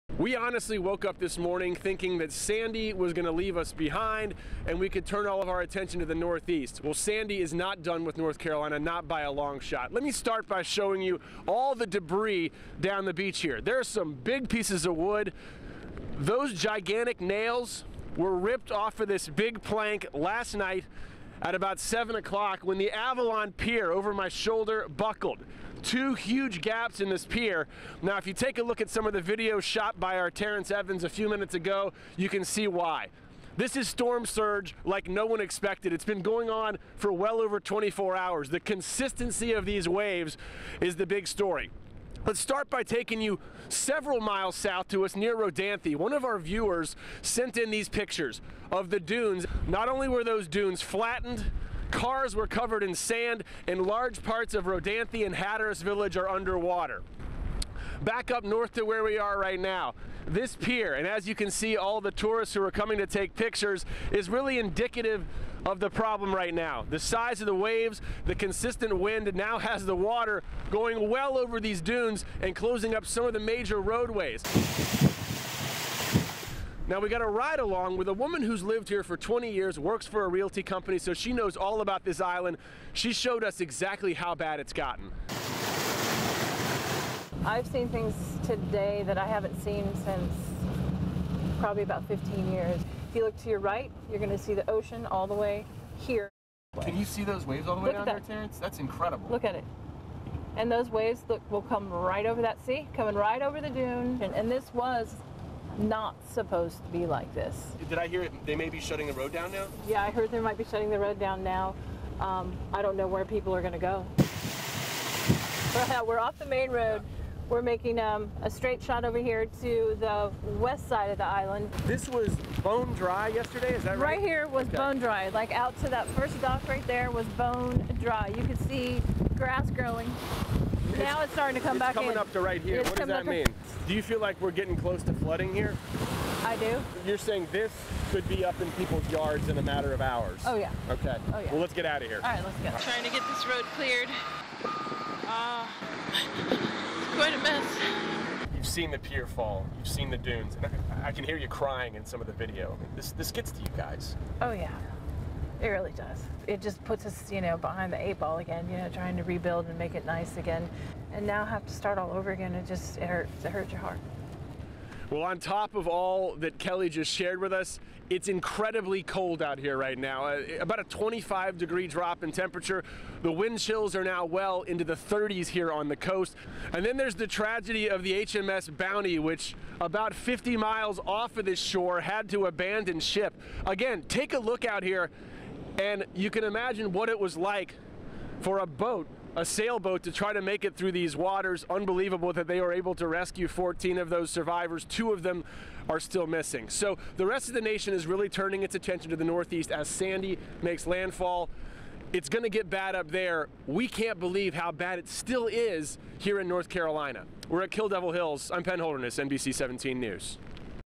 WNCN | Spot News | Hurricane Sandy
Hurricane Sandy rips across North Carolina's Outer Banks, leaving homes, beaches and piers in ruins. A local real estate agent gave NBC-17 anchor Penn Holderness a ride-along to show the flooding and damage caused by the storm.

Reporter: Penn Holderness
Photographer: Terrence Evans